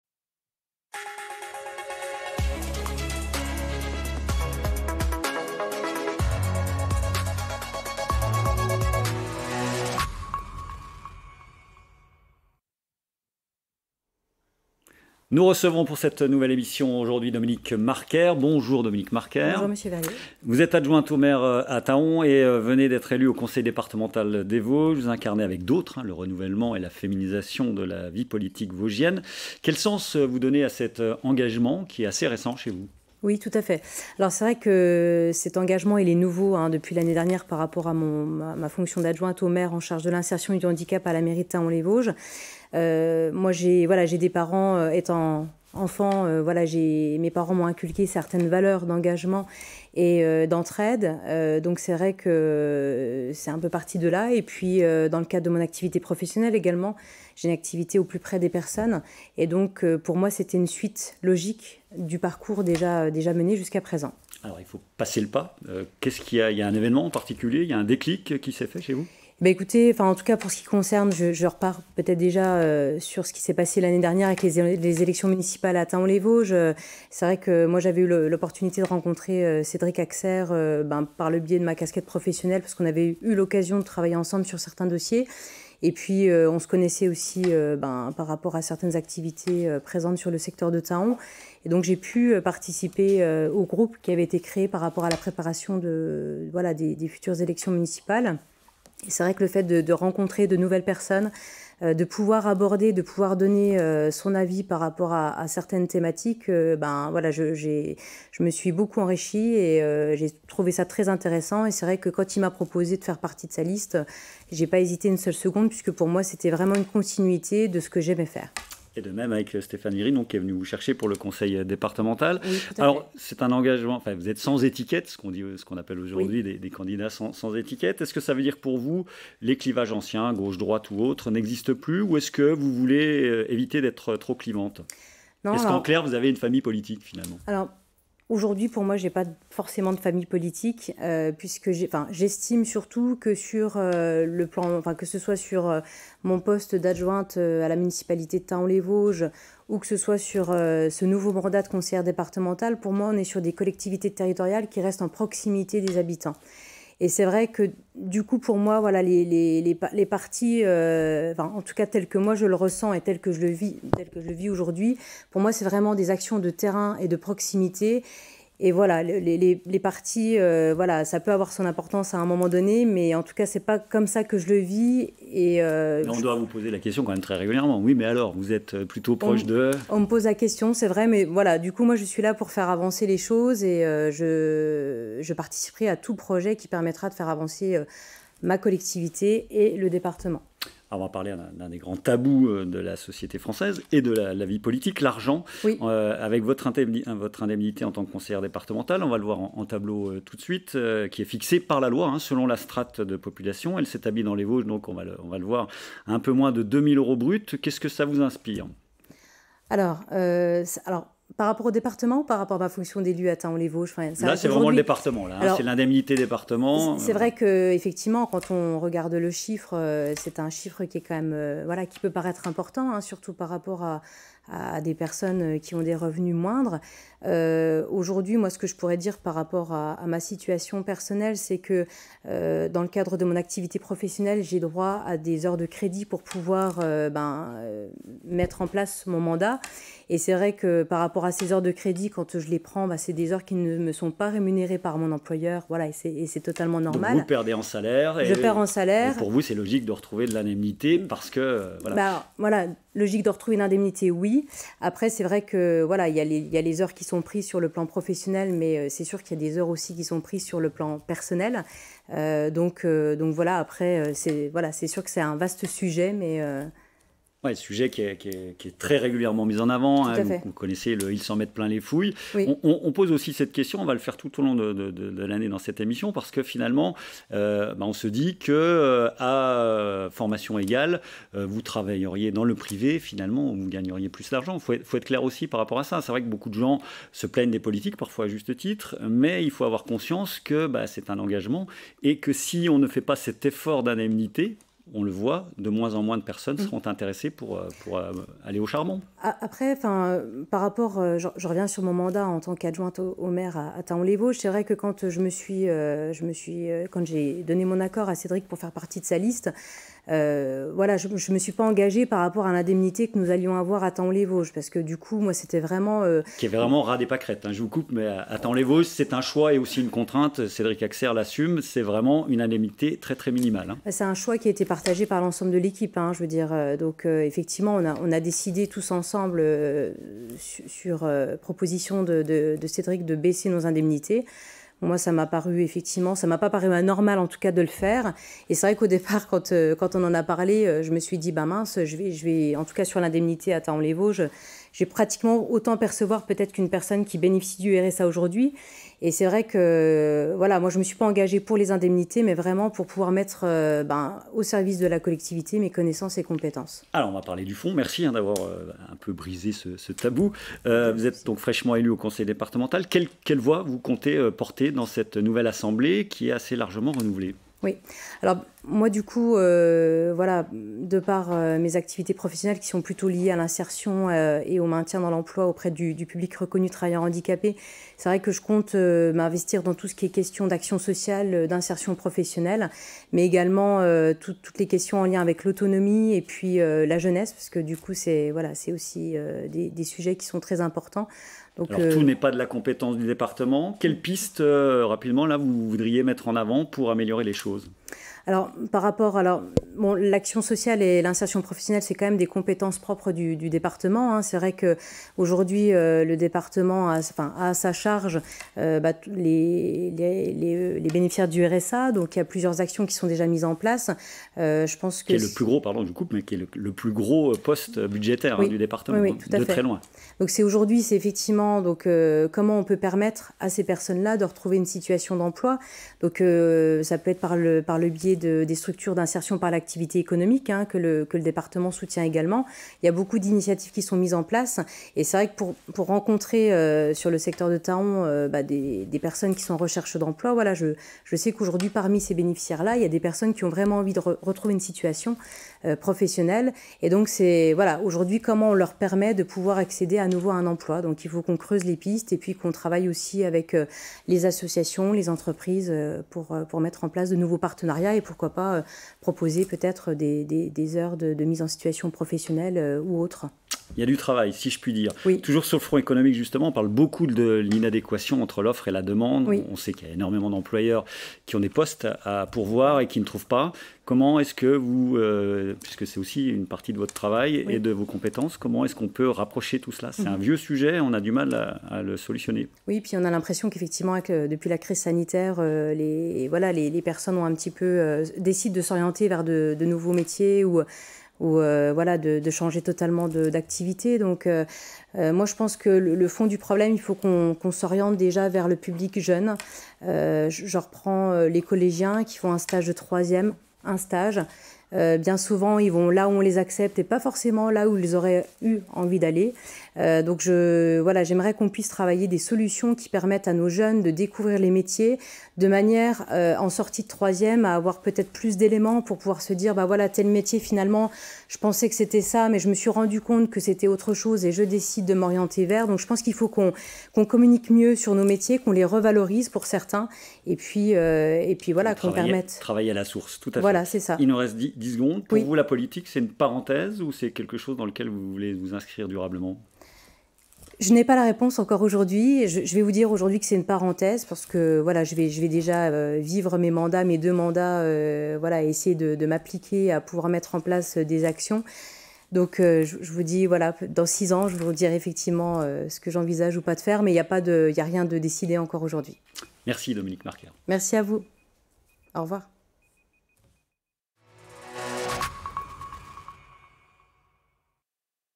Paroles d'élu(e) : Dominique Marquaire
Avec Dominique Marquaire, conseillère départementale (SE), canton de Golbey, adjointe au maire de Thaon-les-Vosges en charge de l'insertion.